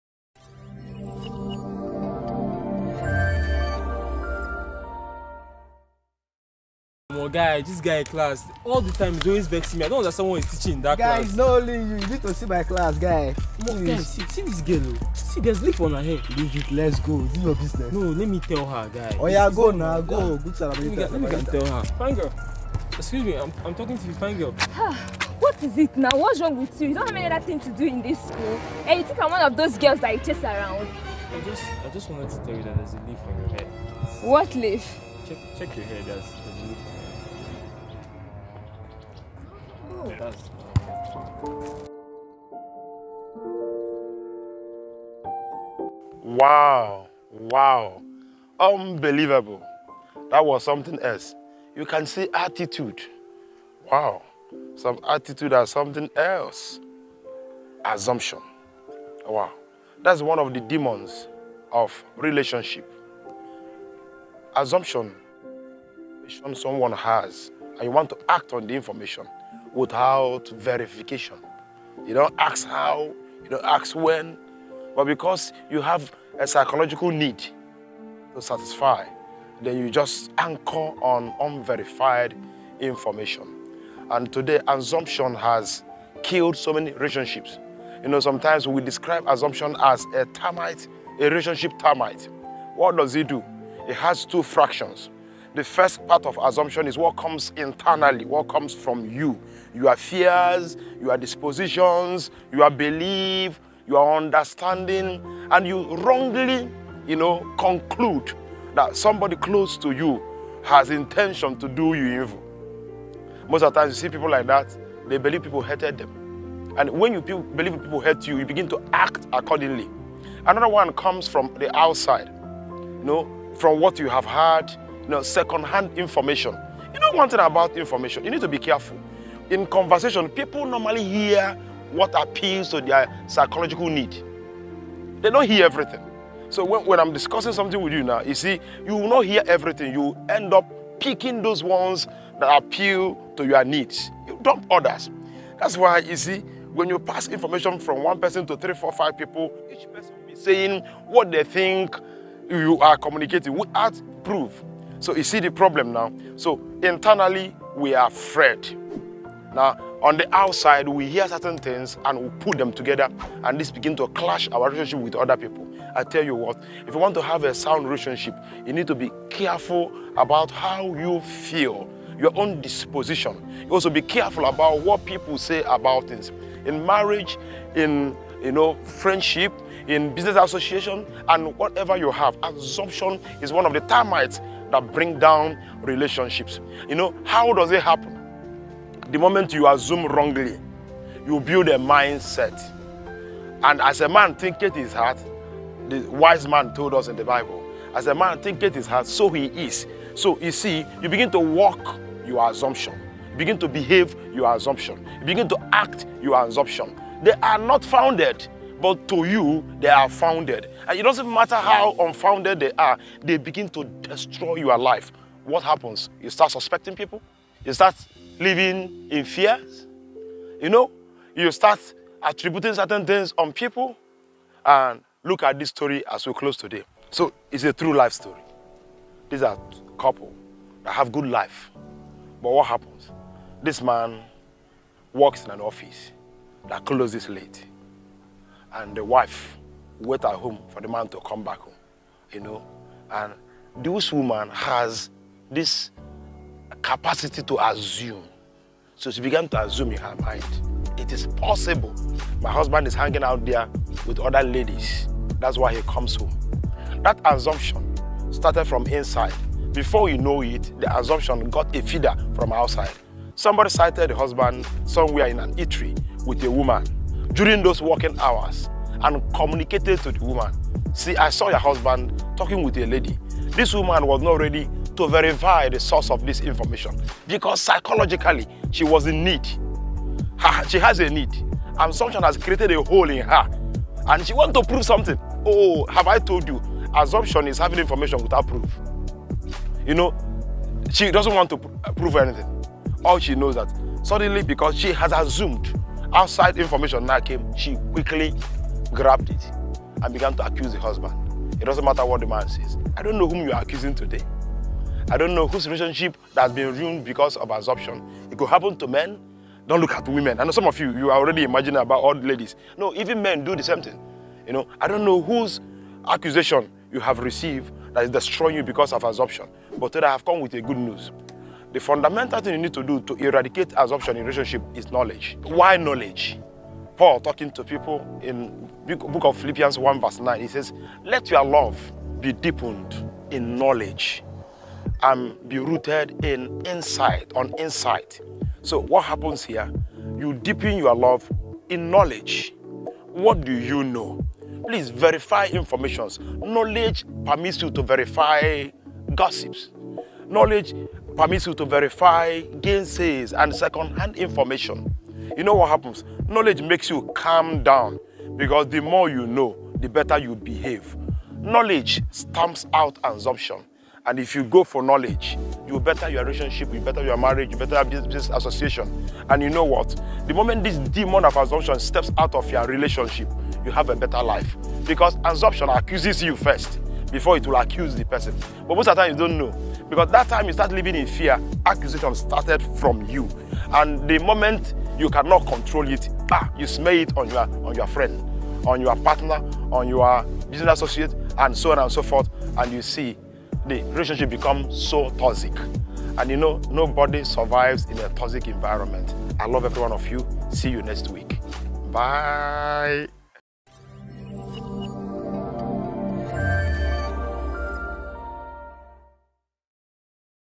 How Assumptions Destroys Your Relationships
ASSUMPTIONS are information acted upon without confirmation.  In this video I critically explained how assumptions can mar your relationship in ways that are unimaginable.  Watch till the end as we fix this issue of Assumption when it comes to relationships.